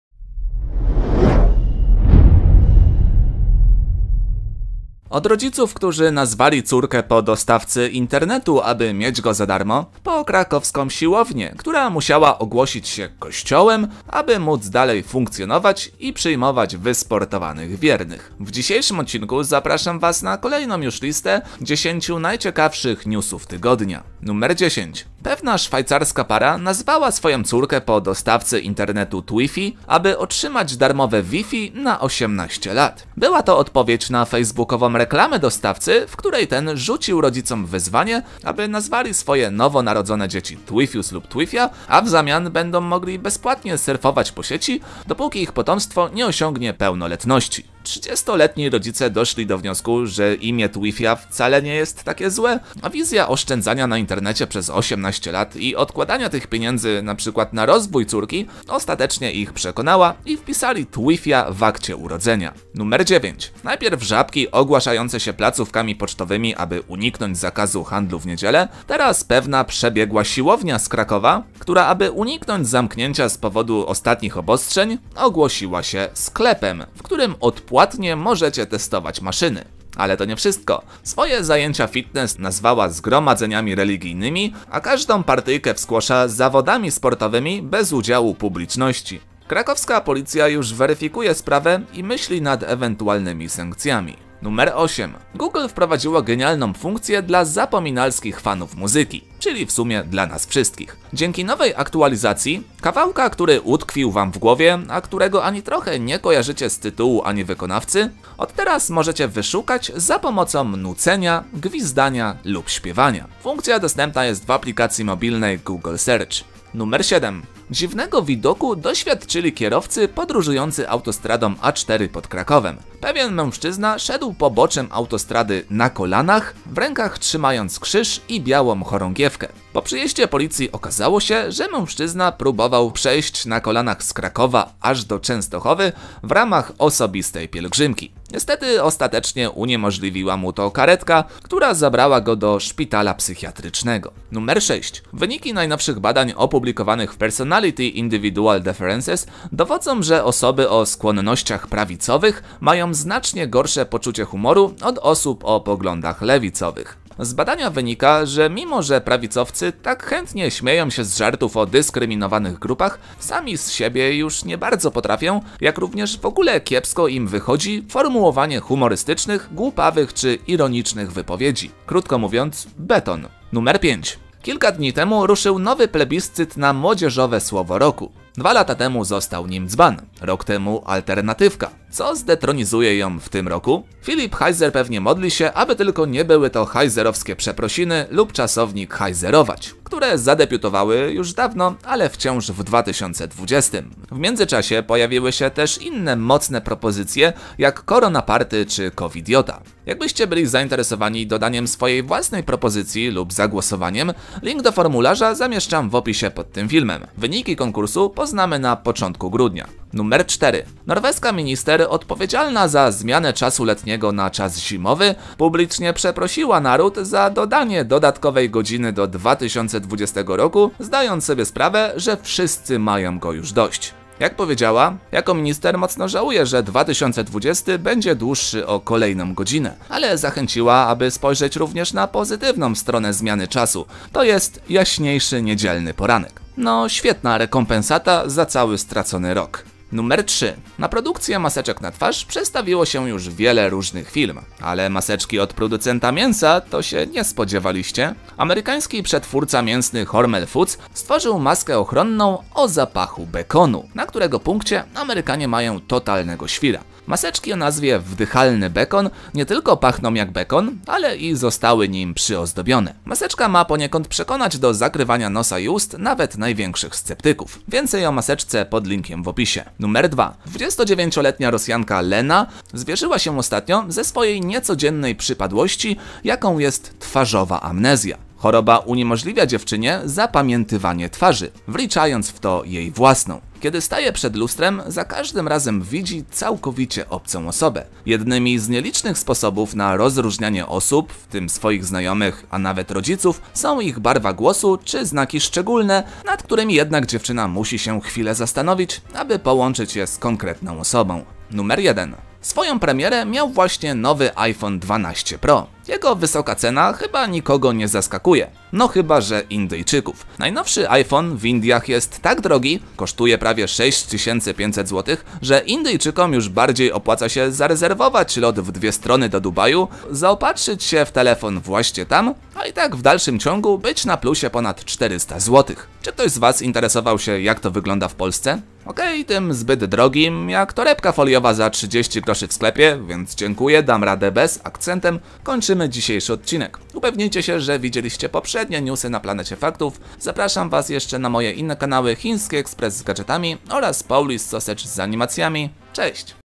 10 najciekawszych newsów tygodnia (25/10/2020)
Od rodziców, którzy nazwali córkę po dostawcy internetu, aby mieć go za darmo, po krakowską siłownię, która musiała ogłosić się kościołem, aby móc dalej funkcjonować i przyjmować wysportowanych wiernych. W dzisiejszym odcinku zapraszam was na kolejną już listę 10 najciekawszych newsów tygodnia.

Źródła informacji, zdjęć i inspiracji dla tego filmu:
10.
9.
8.
7.
6.
5.
4.
3.
2.
1.

Muzyka w tle:
(biblioteka YouTube)

Pozdrawiam,
Jacek Makarewicz